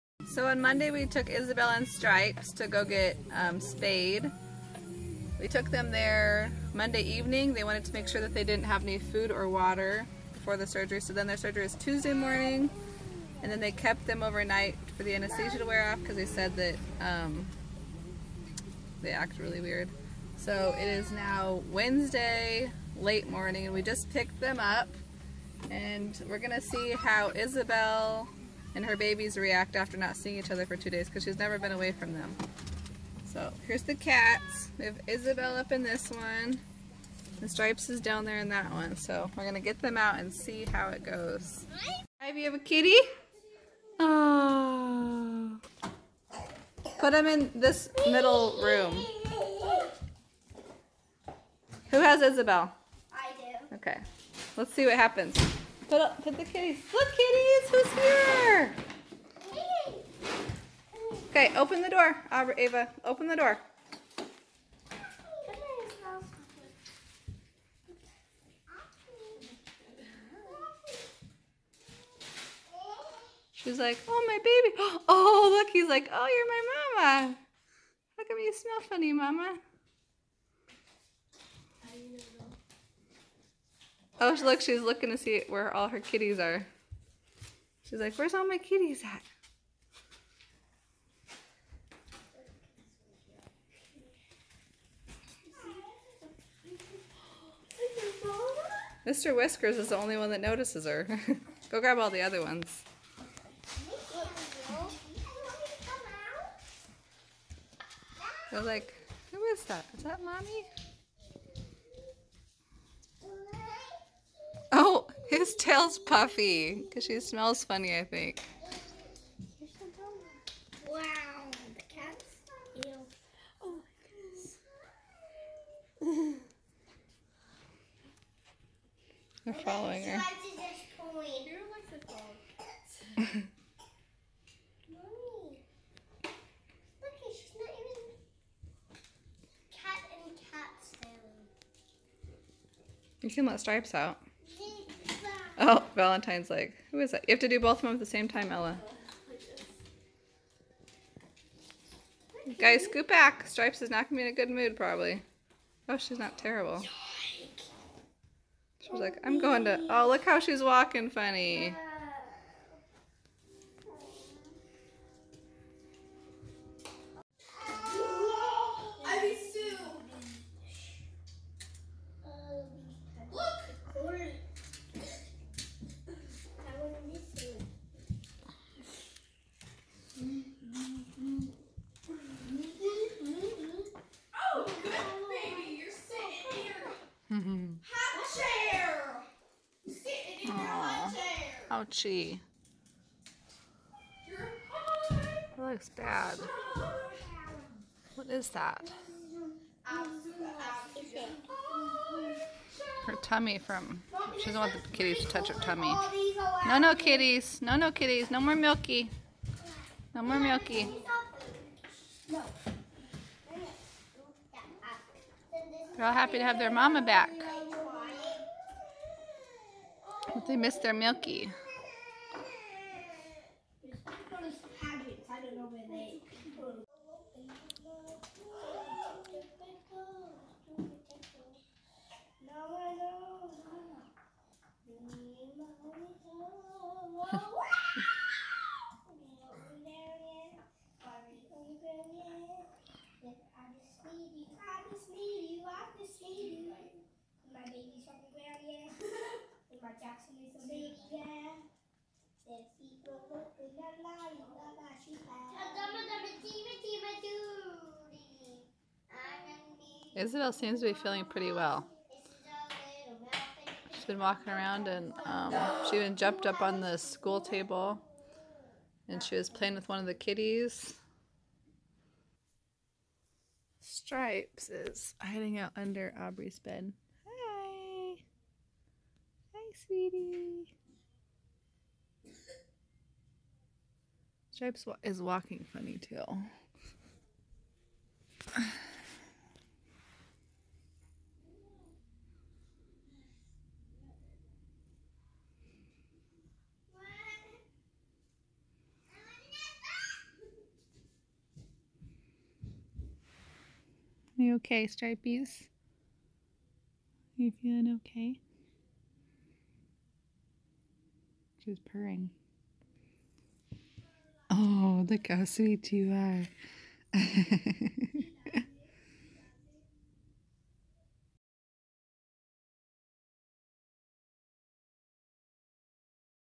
Bringing our cats home after getting spayed!
We had our 2 female outside cats spayed yesterday and we picked them up today!  Isabelle was happy to be home with her 10 week old kitties!